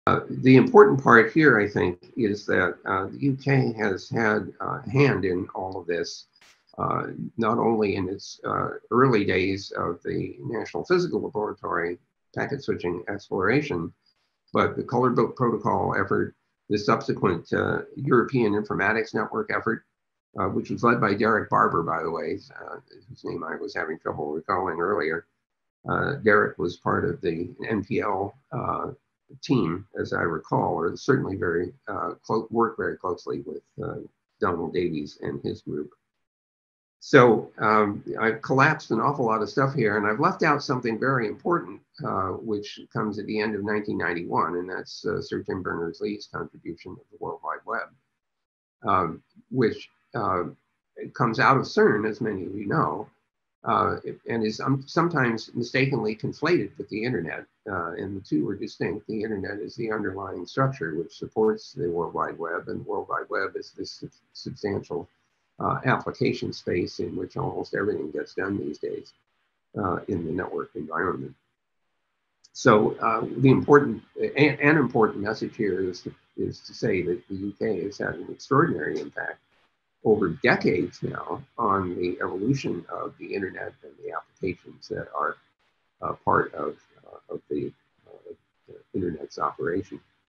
Vint Cerf on the arrival of the WWW
Vint Cerf recalls contributors to the early development of the Internet and the importance of Tim Berners Lee's invention of the www creating new demands for global digital communications